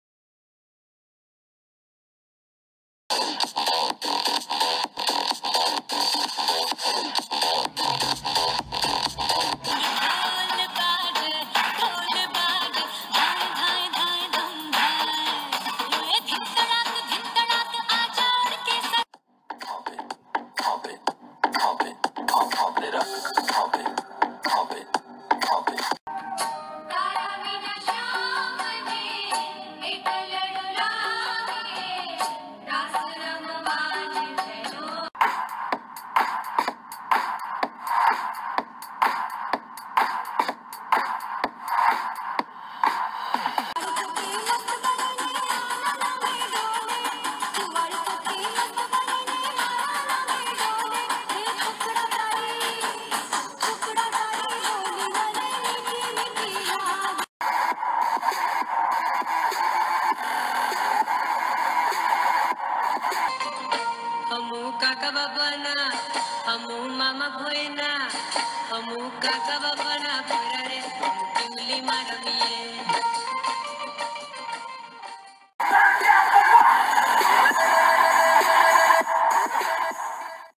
GUJRATIS at GYM V/S Normal people at GYM | FC
GUJRATIS have a different routine altogether for their workout.
This is how GUJRATIS ROLL.
Well, GARBA and GUJRATIS, this bond is surely unbreakable.